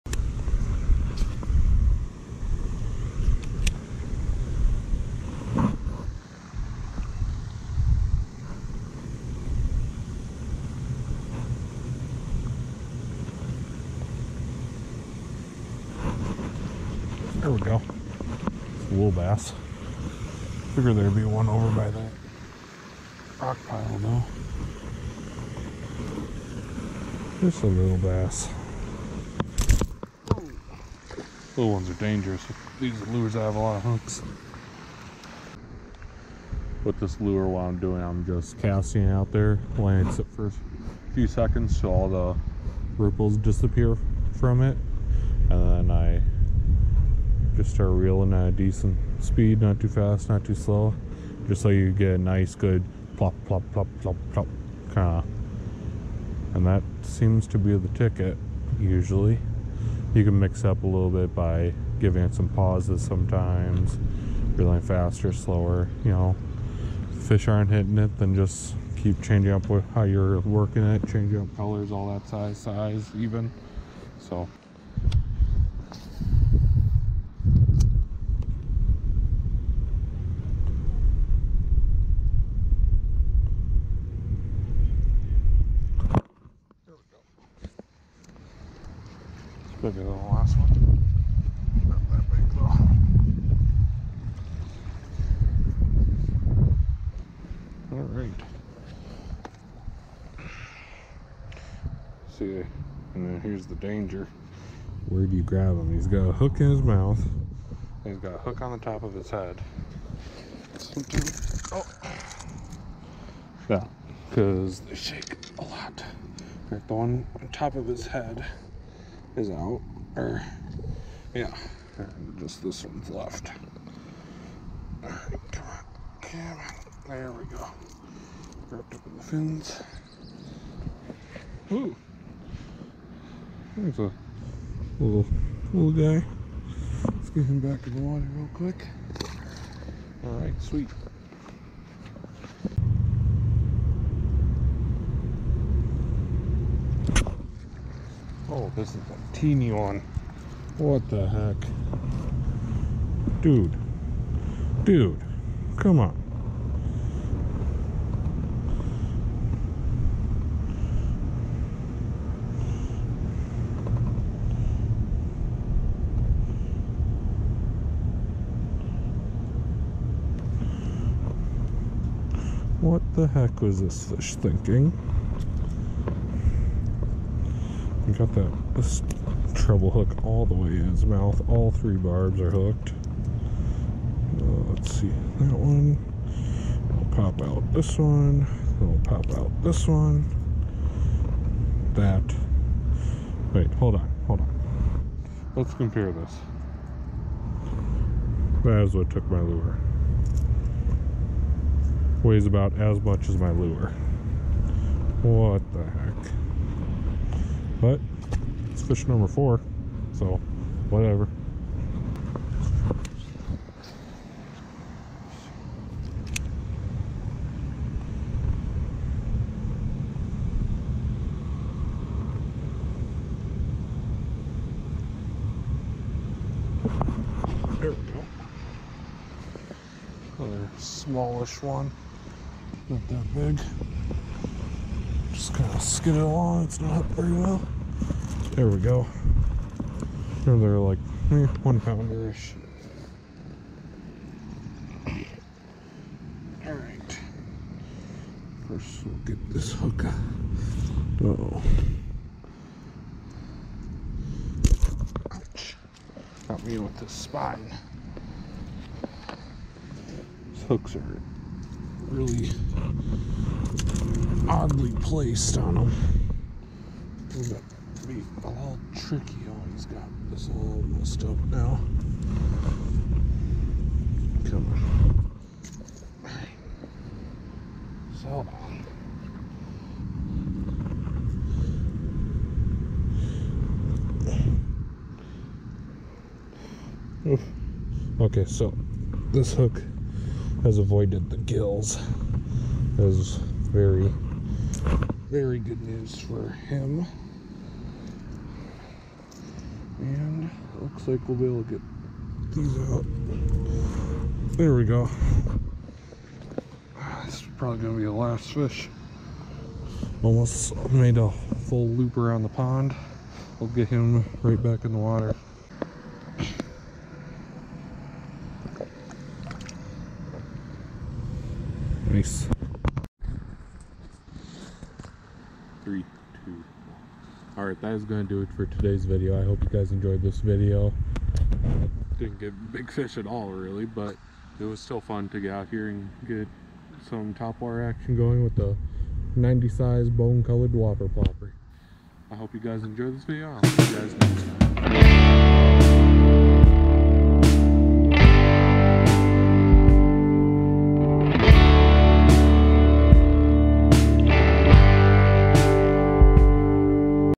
Big Baits for Big Fish? [Wisconsin Fishing]
It seems that bigger baits do not infact guarantee bigger bass!